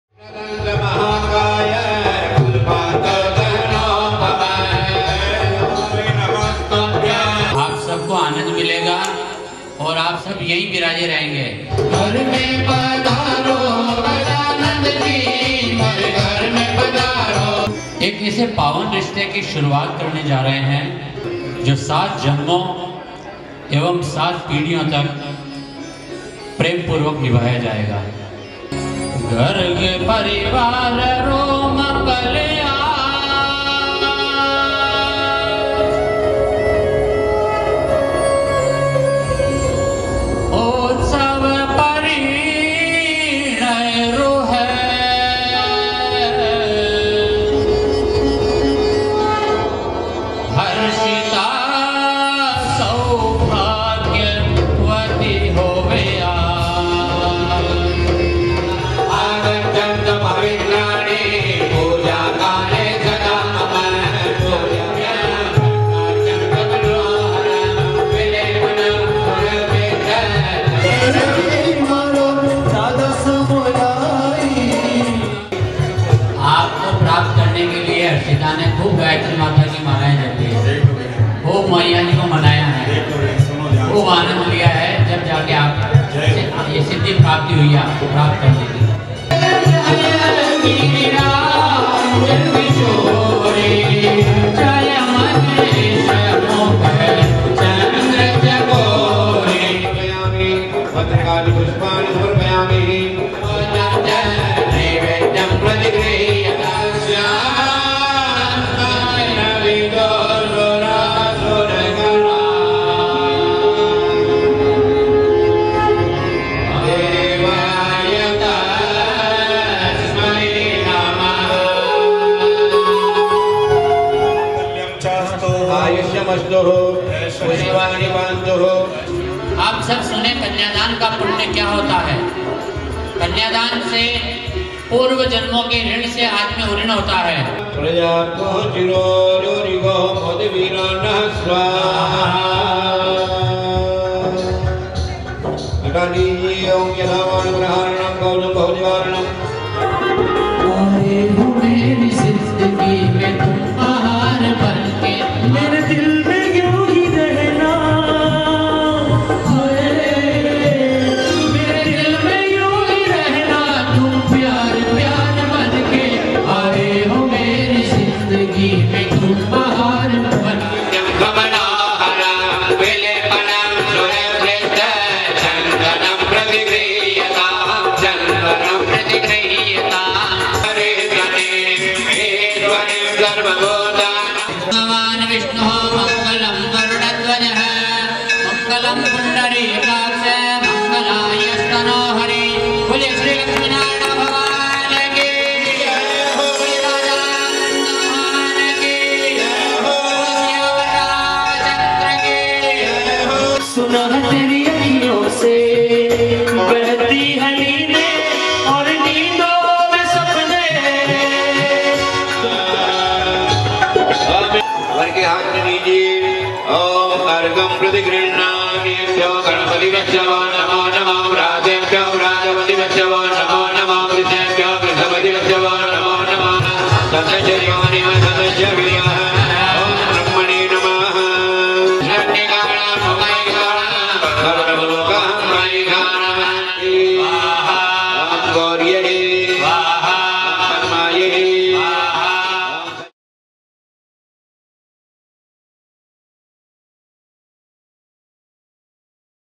Musical Pheras by Pandit Ravi Sharma 9414415287 | Jain Musical Shaadi
Book Pandit Ravi Sharma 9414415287
आजकल के परिपेक्ष्य में विवाह संस्कार में व्यक्ति अपने सामर्थ्य के अनुसार नए से नए इवेंट्स करवाता रहता है उसी  थीम को ध्यान में रखते हुए YouTube के पंडित जी रवि शर्मा ने एक नया आयाम जोड़ा है संगीतमय परिणय संस्कार  Musical Pheras.
जब पंडित रवि शर्मा जी म्यूजिकल फेरा प्रारम्भ करते है तो विवाह में आये हुए आपके घराती और बाराती सभी इस संगीतमय परिणय संस्कार को देख,वहां से हिल भी नहीं पाते है अर्थात एसा आनन्दमय वातावरण बन जाता है